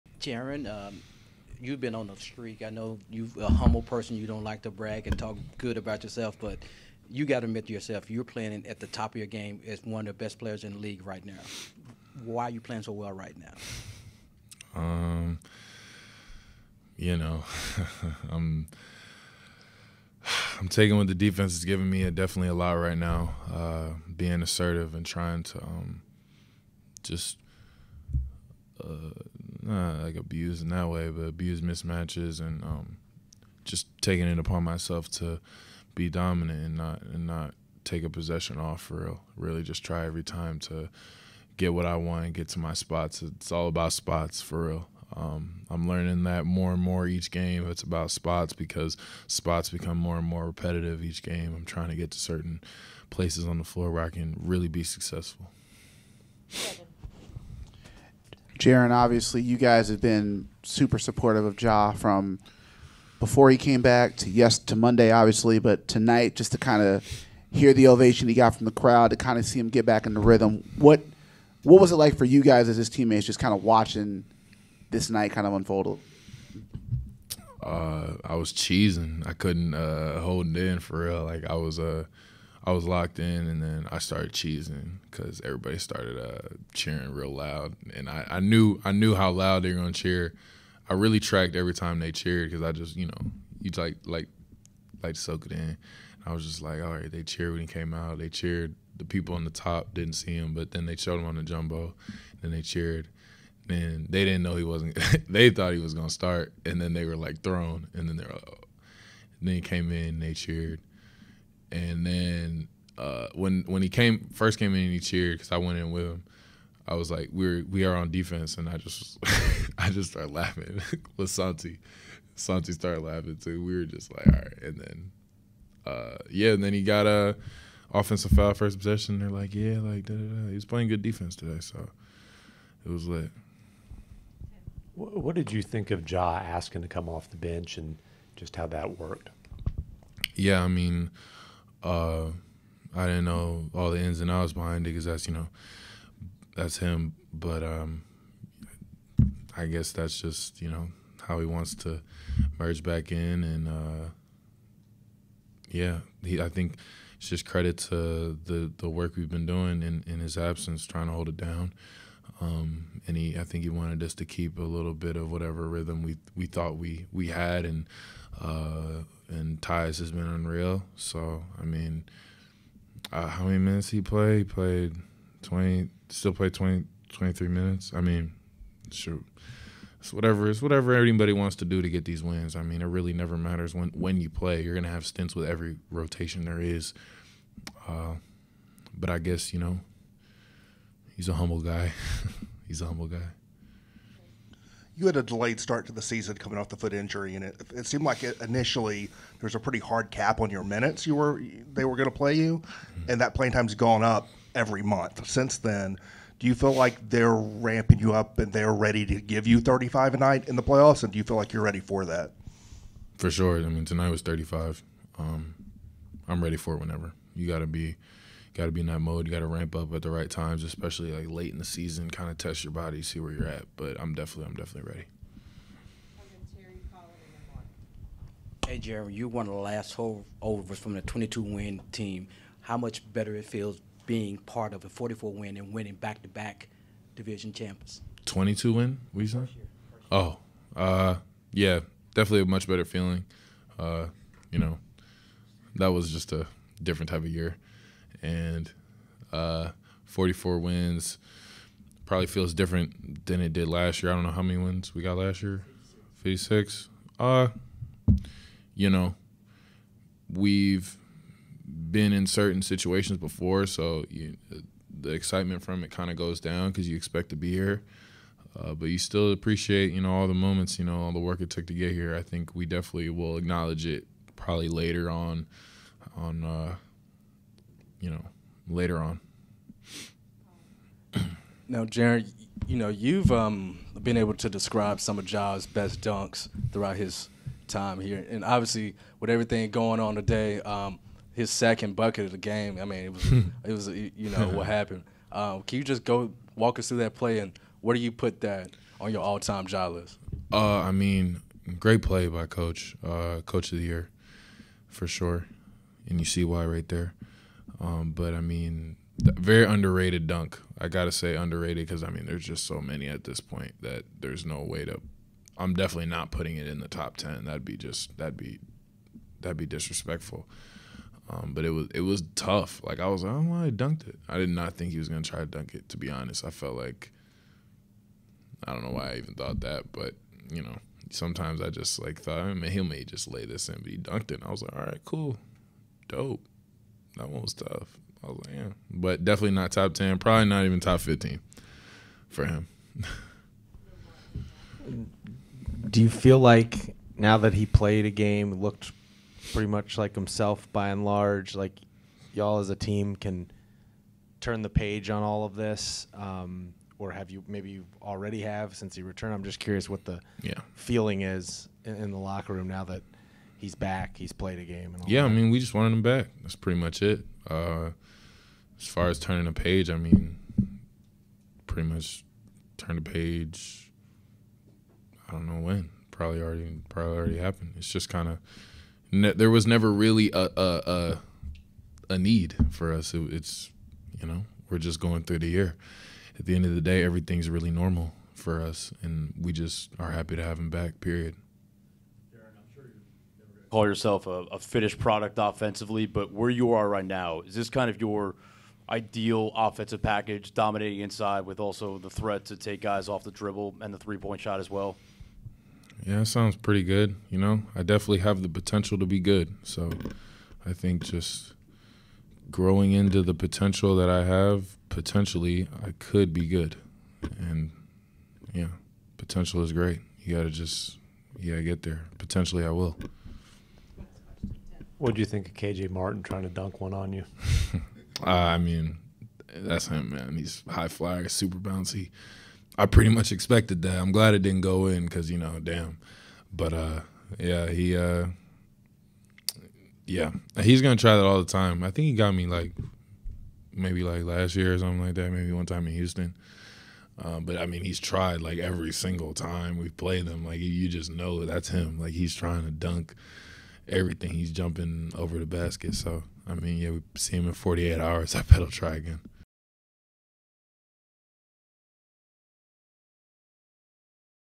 Jaren Jackson Jr Discusses His High Level Play: "I'm trying to abuse mismatches"
Jaren Jackson Jr joined the media after the Grizzlies' 130-125 win over the Houston Rockets.

He discussed his high level play, the return of Ja Morant and being tested at the rim by Kenyon Martin Jr.